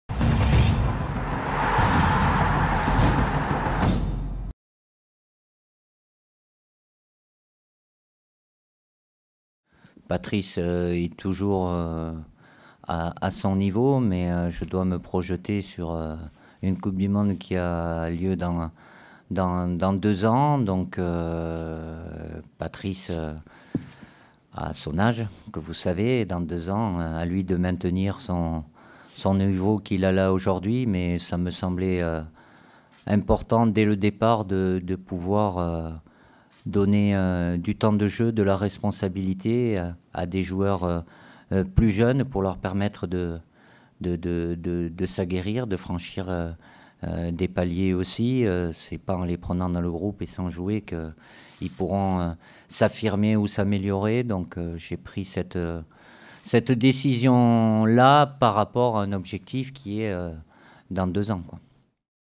Deschamps justifie l'absence de Patrice Evra dans la liste des 23
Didier Deschamps n'a pas retenu Patrice Evra dans la liste des 23 des Bleus et s'en est expliqué lors de sa conférence de presse.